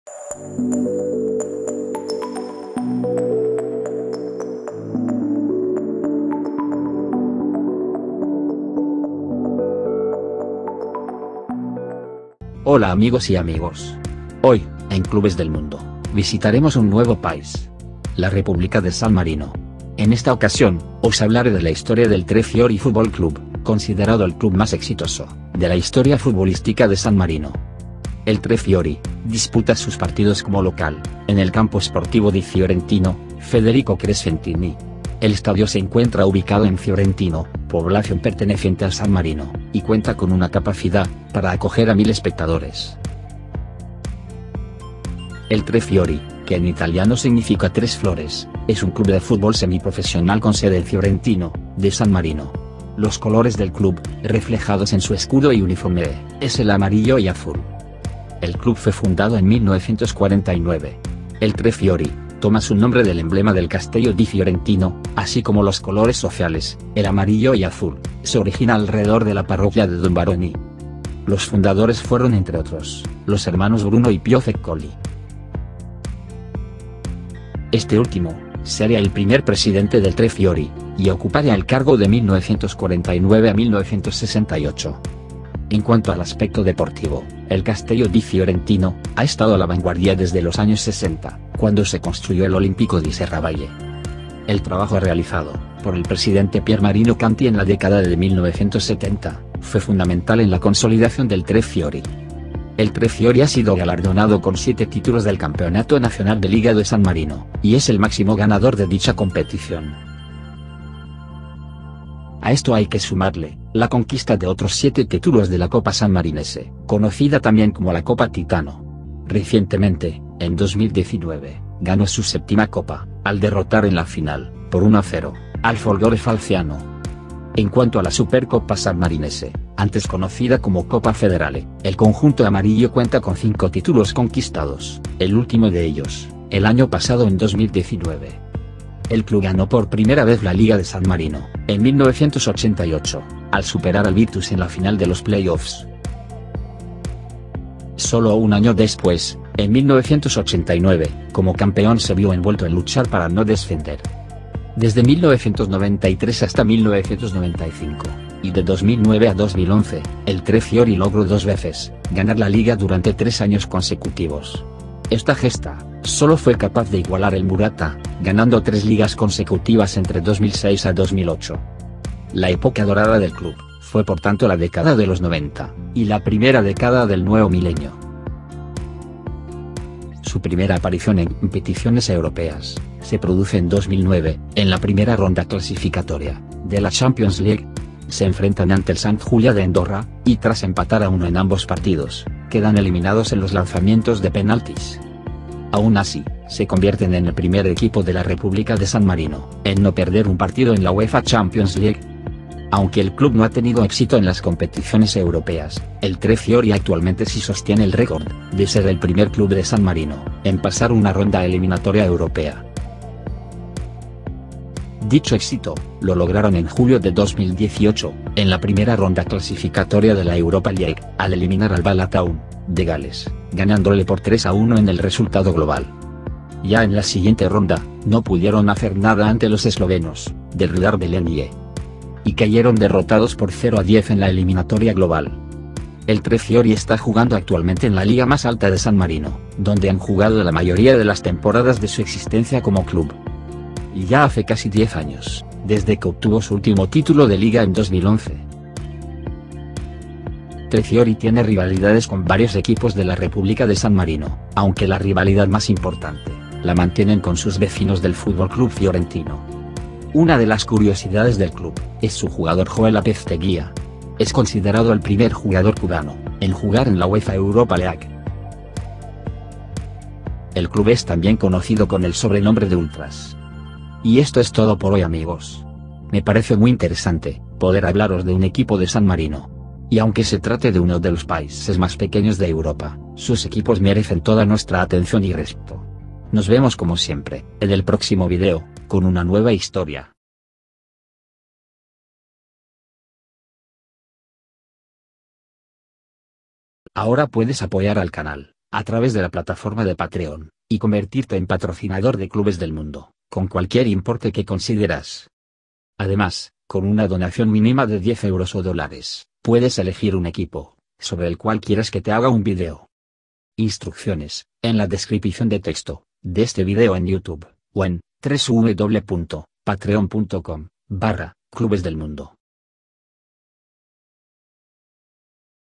TRE FIORI - El Club más laureado de San Marino y primero de S.M. en pasar una Eliminatoria Europea
Conoce en este video la Historia del Tre Fiori. Es el club más laurado de la República de San Marino, con 7 titulos de Liga conseguidos.

Además, en 2018, fue el primer club de San Marino en pasar una eliminatoria de Competición Europea. Hazaña que lograron ante el Bala Town de Gales.